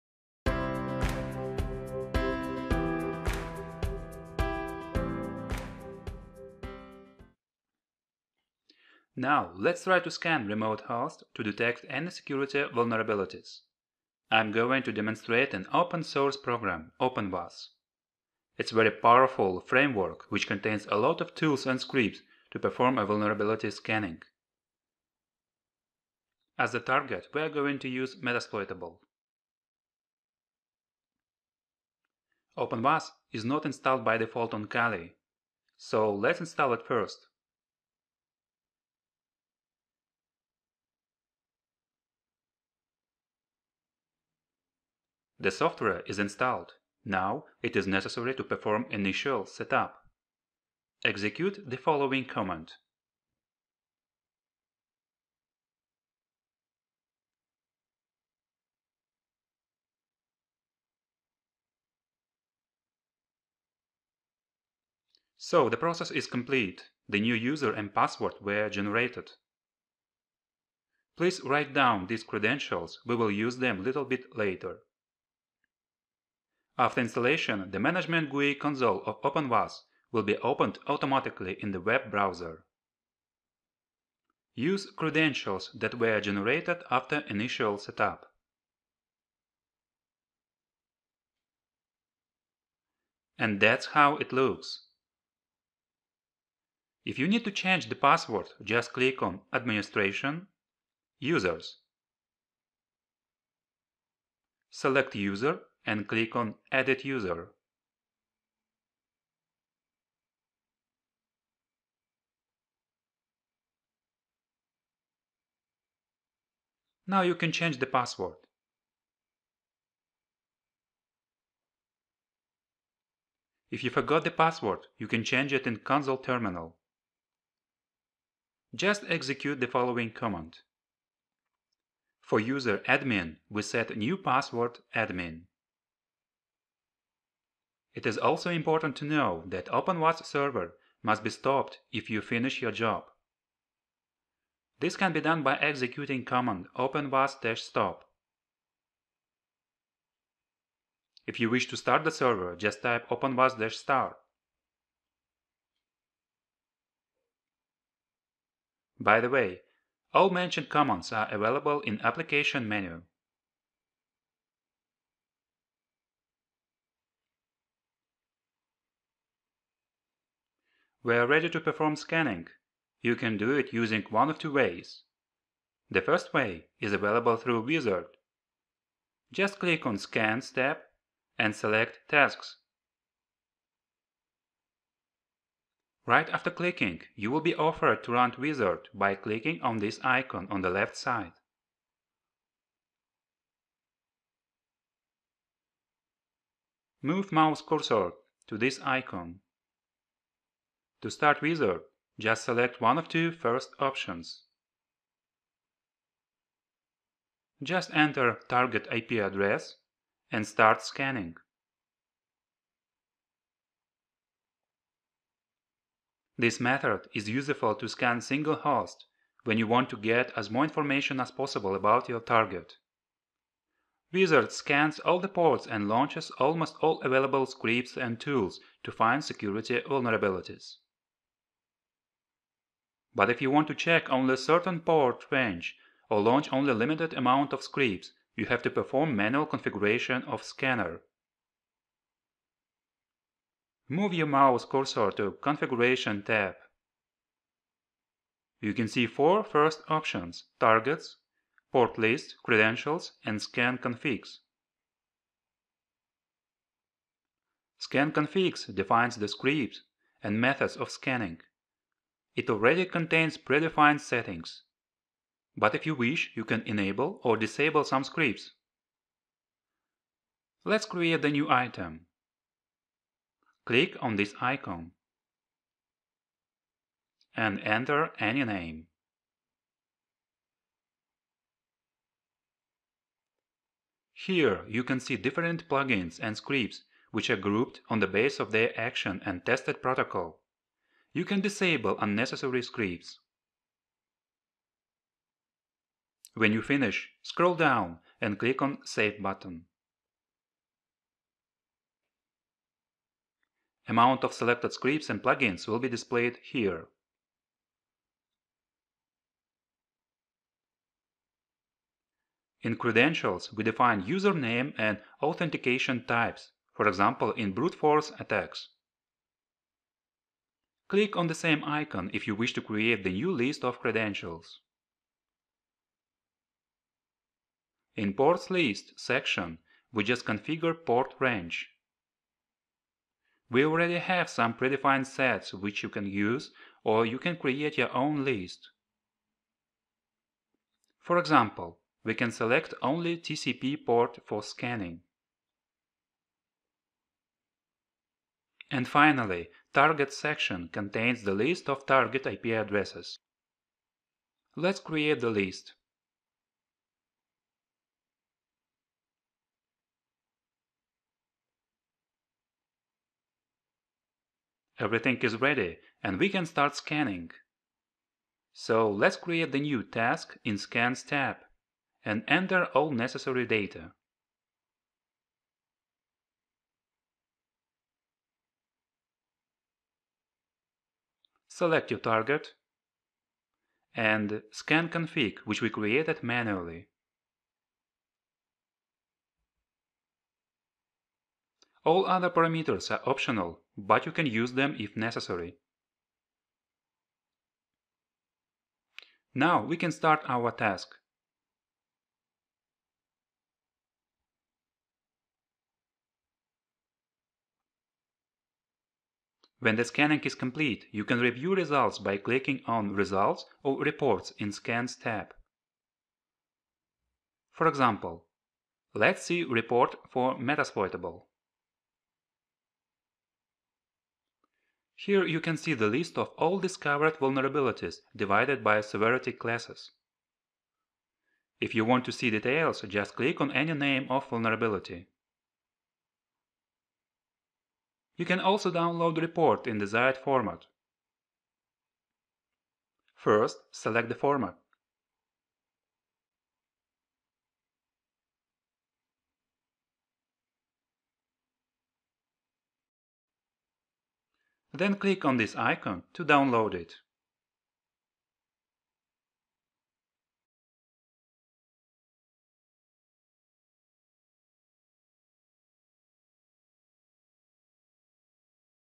OpenVAS Tutorial | Setup | Vulnerability Analysis and Scanning
Learn how to utilize OpenVAS effectively for website vulnerability scanning, including scan customization and target configuration.

OpenVAS is open source project that can be easily installed and run on Kali Linux. This tutorial provides a complete guide for penetration testers.